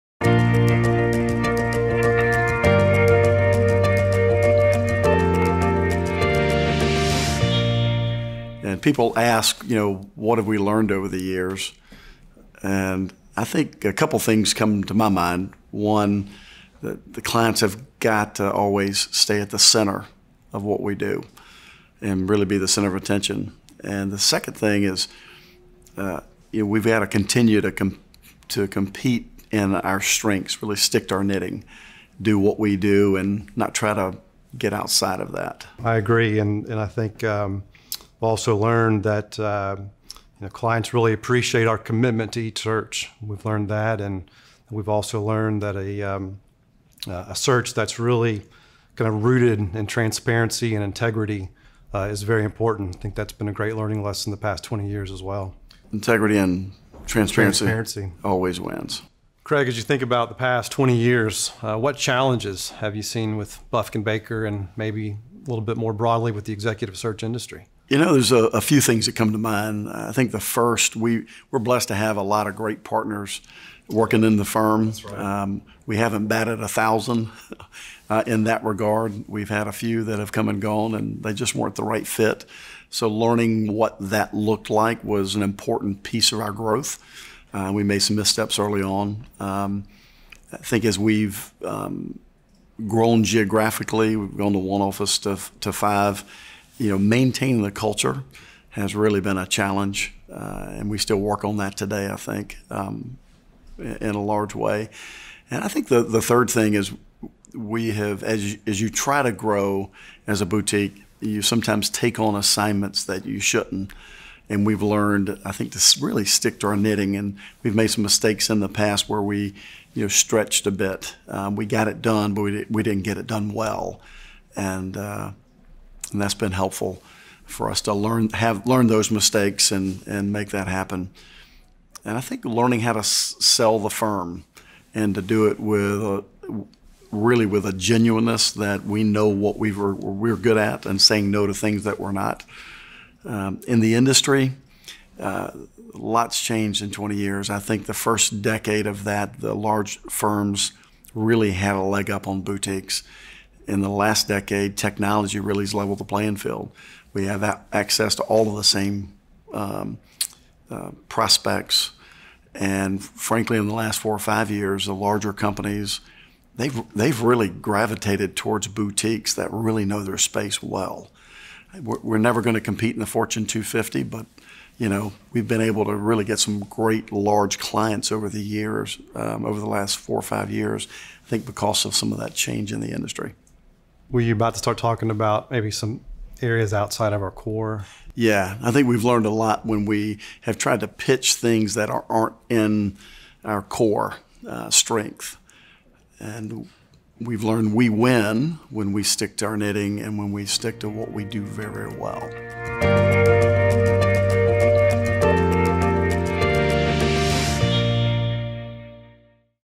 Bufkin / Baker and the History of Our Firm
Buffkin / Baker is a partner-led executive search firm focusing on healthcare, higher education, private equity, entertainment/media, digital, and non-profit.  As leaders in their respective fields, each partner has over a decade of retained search experience. Combined, the firm's partners have more than 175 years of search experience and more than 220 years of industry/operating experience. Buffkin / Baker is a member of Panorama, a global community of over 400 leadership experts working together to promote a diverse perspective of leadership, embracing creative thinking, and sharing our expertise and experience to bring our clients the very best results.